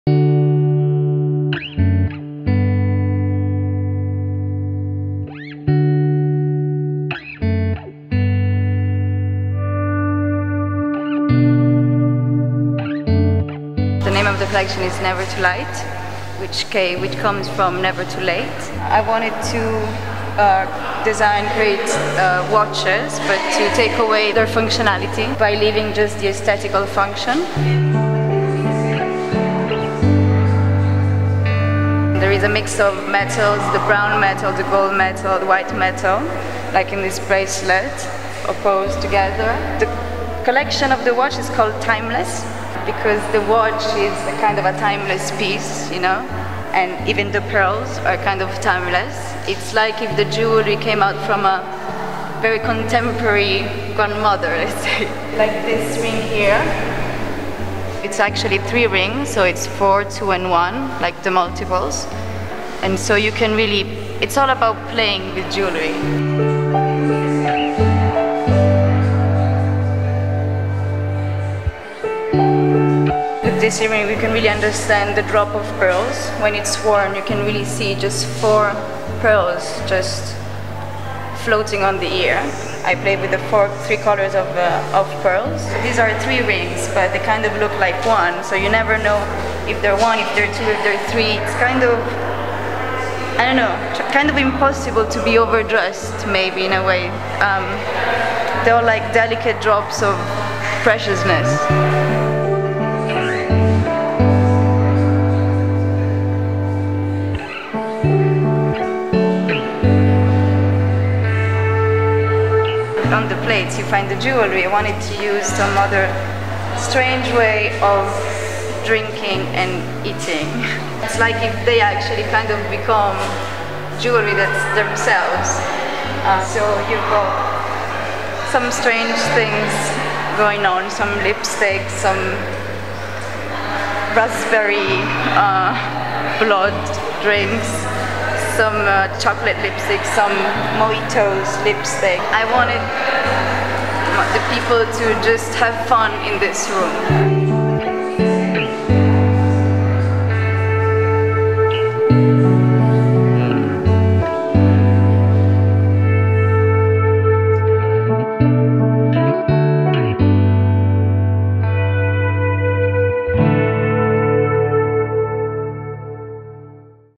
Delfina Delettrez at Almine Rech Gallery
Rome-based jewelry designer Delfina Delettrez took over the Almine Rech Gallery in Paris for the recent presentation of her collection, "Never Too Light," playing on the theme of gravity and illusion, with the pieces designed to appear to float.